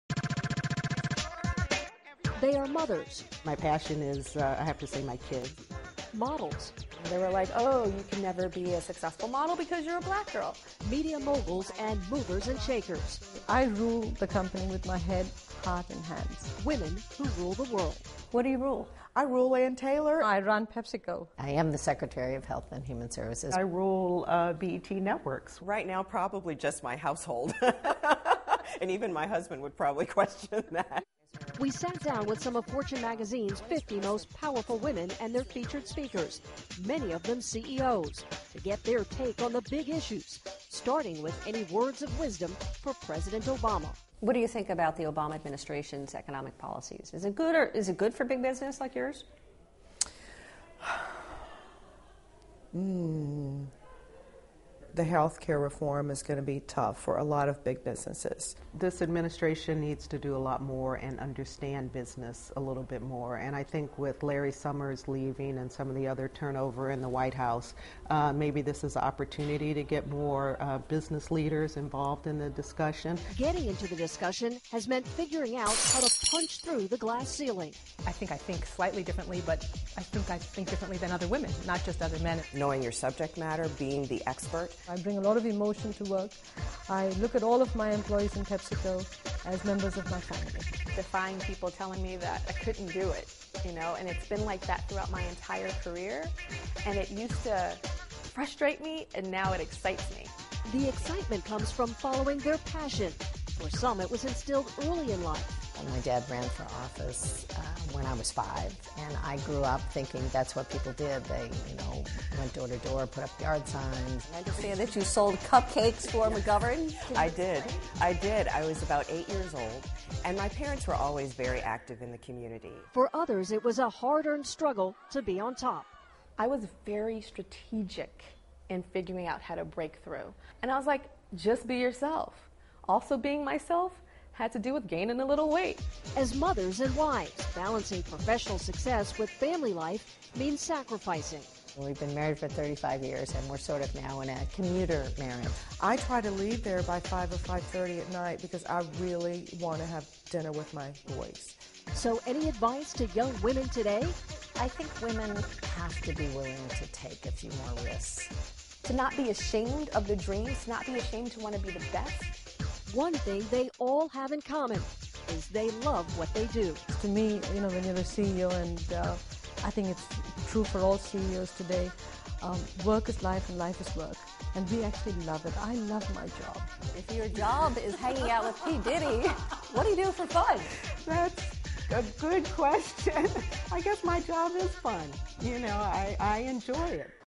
Powerful women on Obama
Some of Fortune's most powerful women explain what motivates them and sound off on President Obama?s policies.